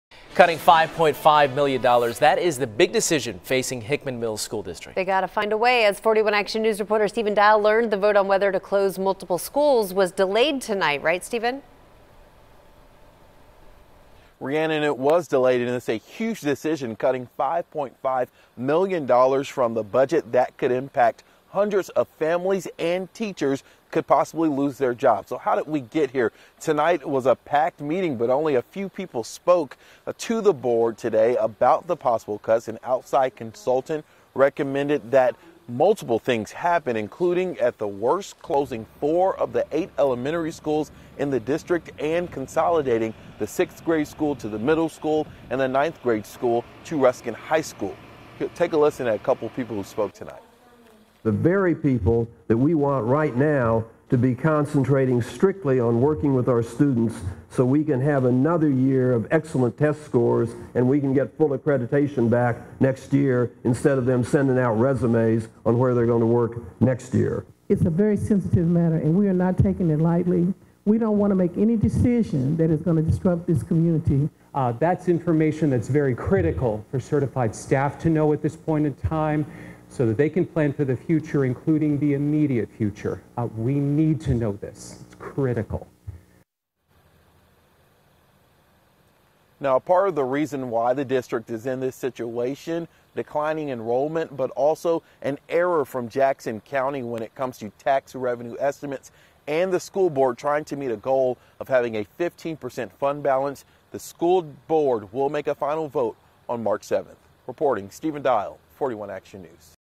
Possible Hickman schools to close, vote March 7
The Hickman Mills School board delayed a vote to cut $5.5 million dollars from its budget, possibly closing multiple schools.